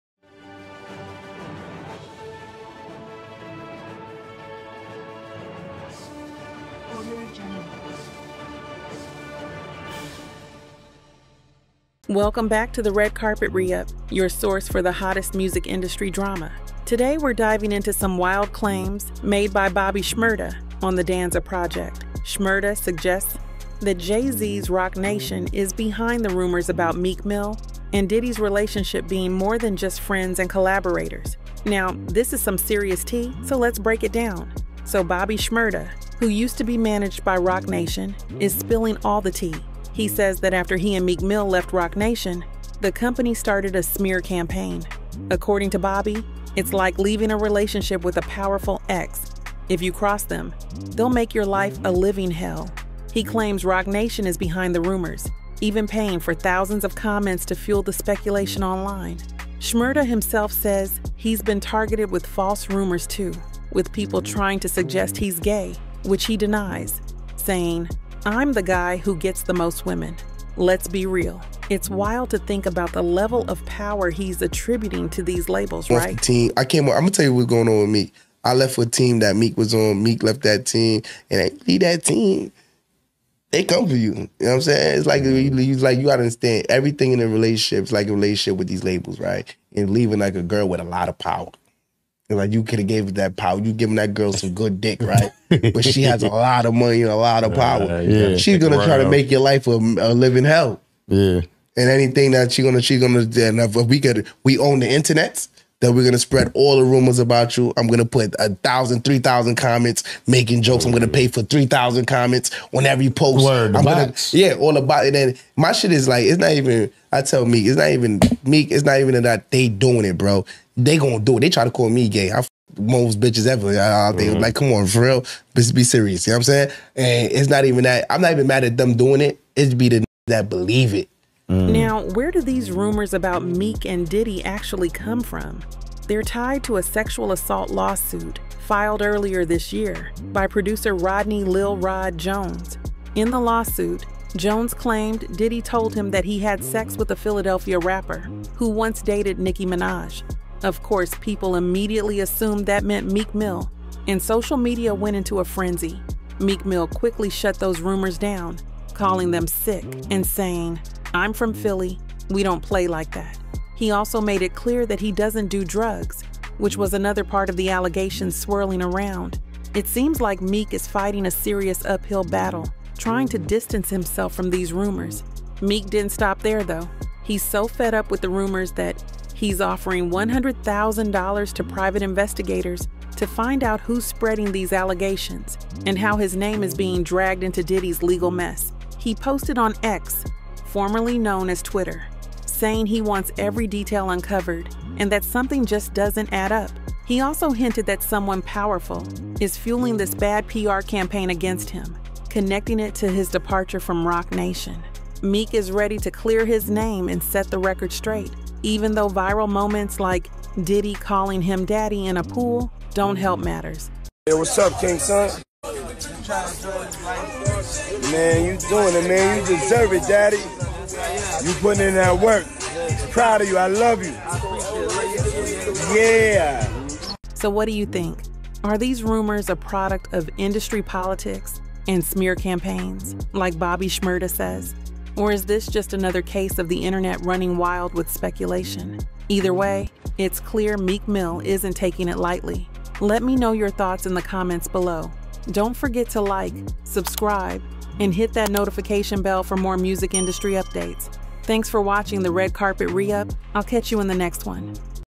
Bobby Shmurda on Roc Nation's Gay Rumors About Diddy & Meek Mill
In this video, I'm going to break down Bobby Shmurda's explosive claims about Roc Nation spreading gay rumors involving Diddy and Meek Mill. Bobby Shmurda Roc Nation gay rumors are at the center of this controversy, with Shmurda suggesting that Roc Nation planted these stories after Meek Mill left their management.
Throughout this video, we'll also cover Meek Mill's denial of these claims and how the rumors have spread, including Bobby's shocking interview. Is Roc Nation really behind these stories, or is it just internet hype?
Get more details as I unpack the drama and give you the inside scoop.